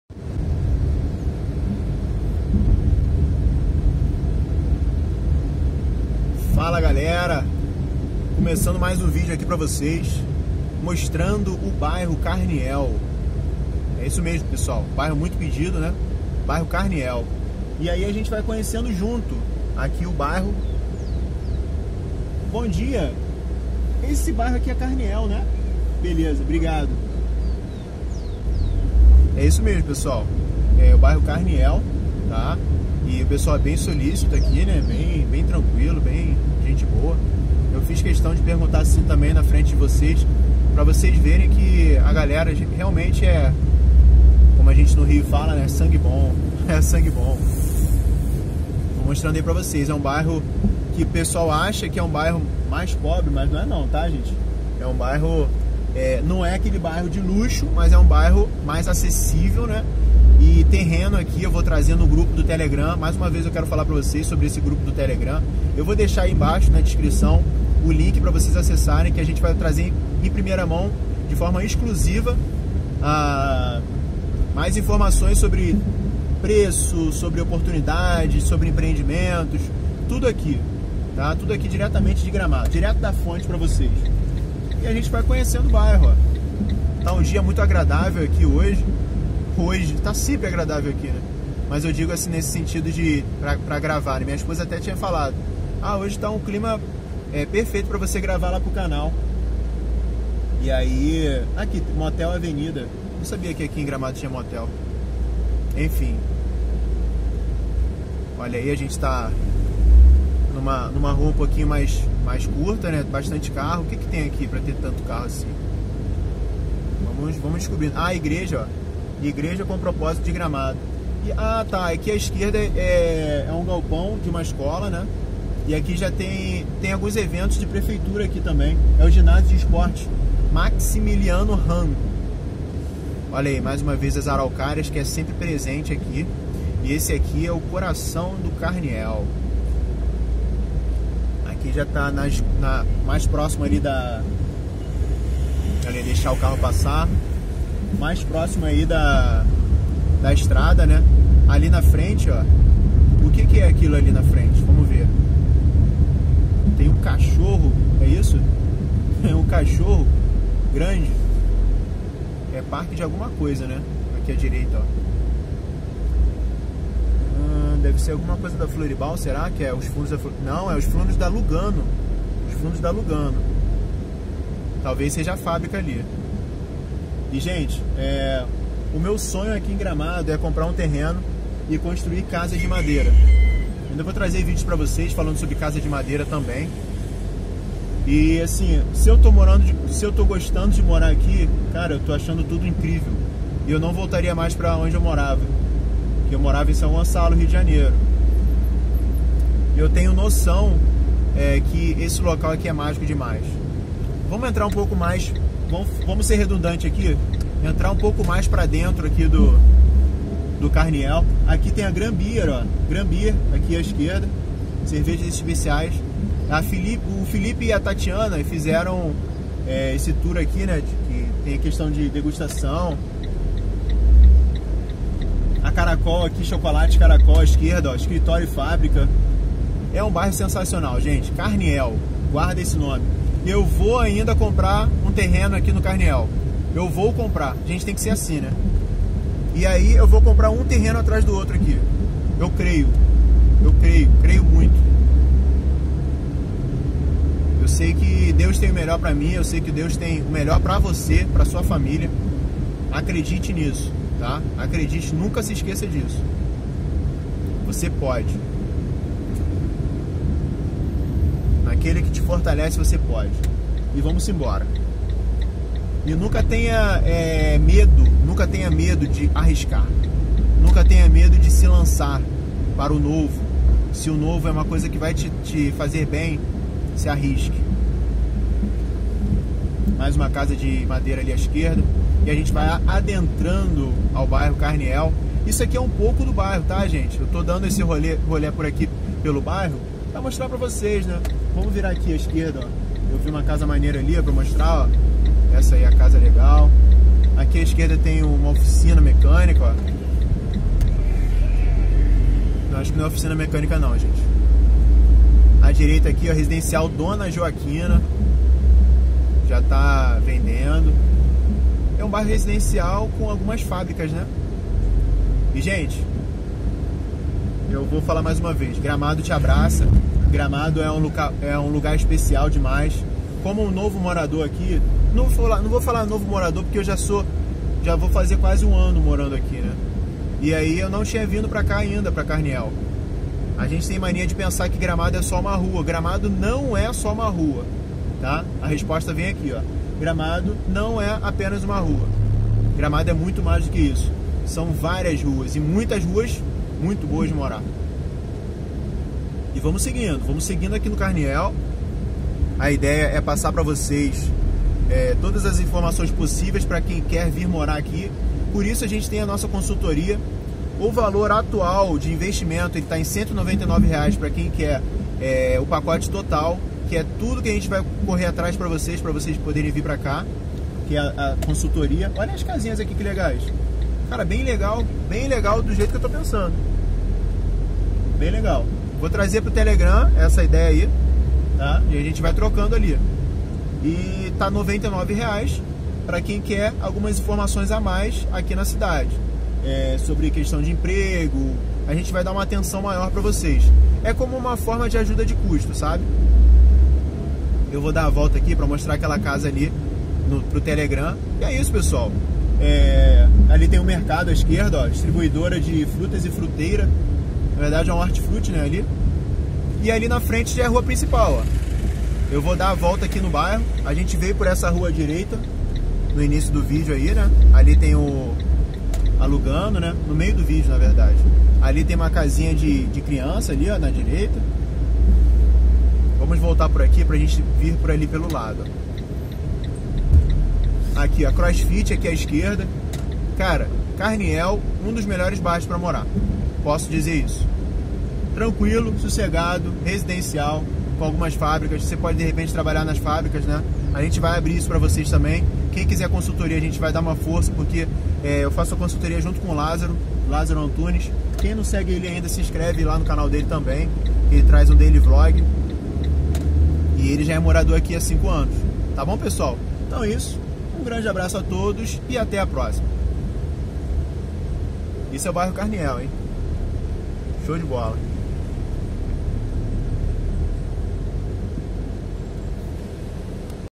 SE EXISTE UM BAIRRO PERTO DE GRAMADO CENTRO E CANELA AO MESMO TEMPO | Bairro Carniel
Fui pela primeira vez no Bairro Carniel em Gramado, um dos bairros mais pedidos por vocês aqui no canal.

E saiba mais sobre como funciona nossa consultoria.